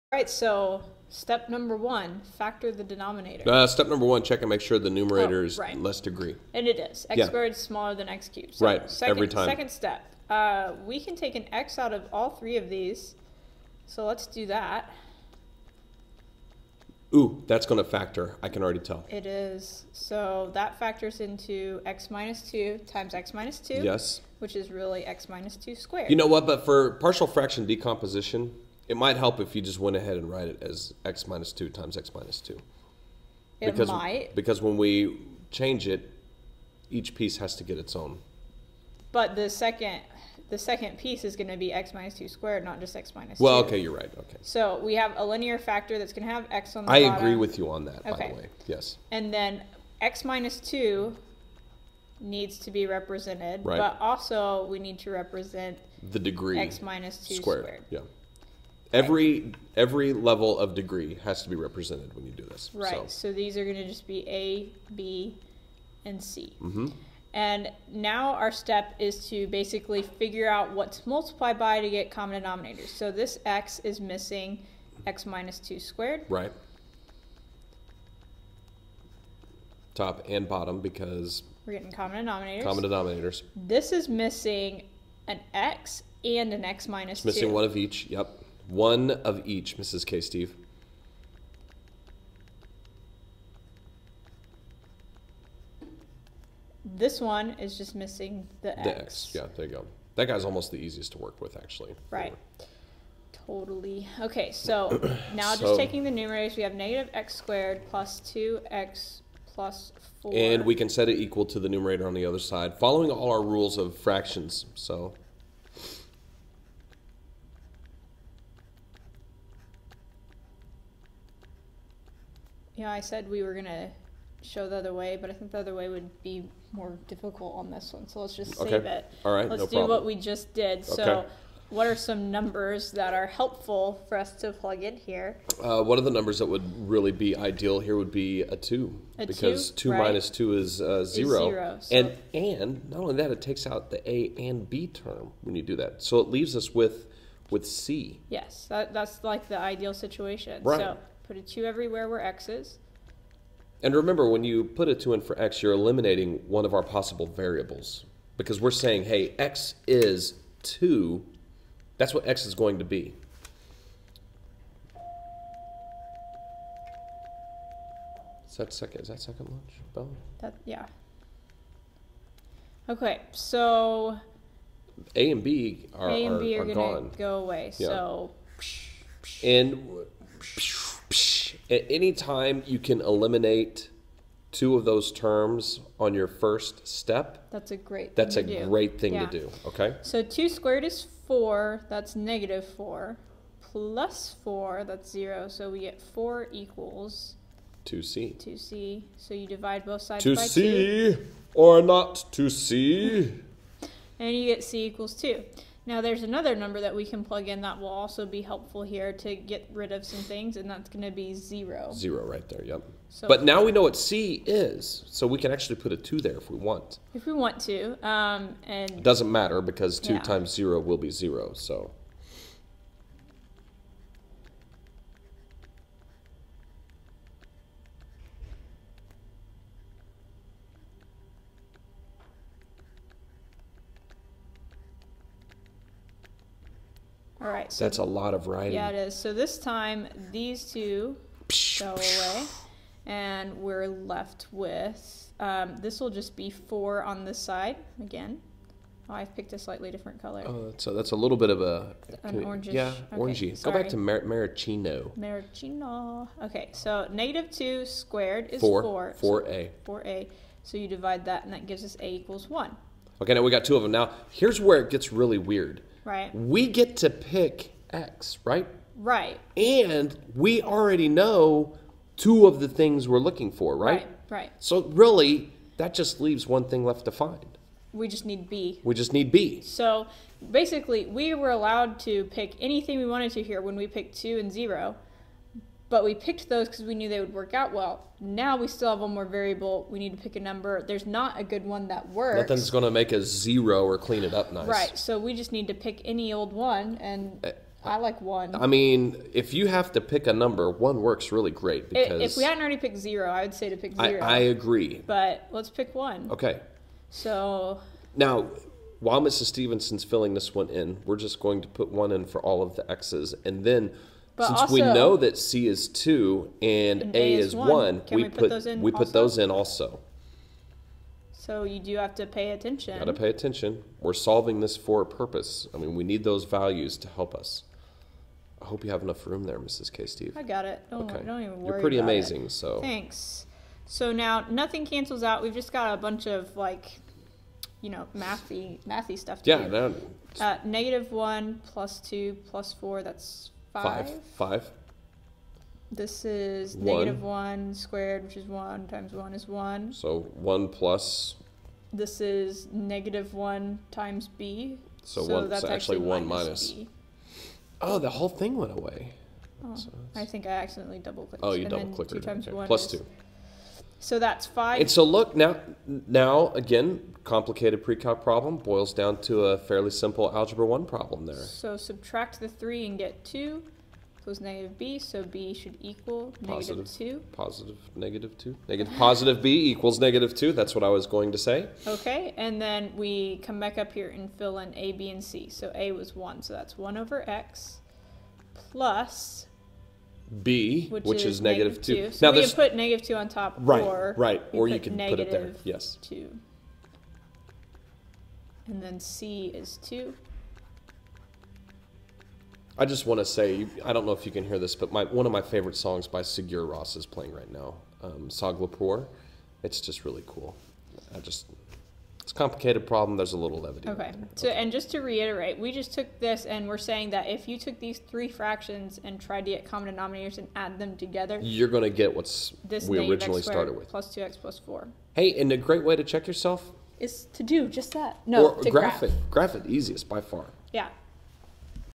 Precalculus - 7.4 Notes Example 3: Solving a Partial Fraction Decomposition Problem Example 2
This time we will solve the partial fraction decomposition problem all the way down to the fractions.  There are several different ways to do this and we will show you two of them.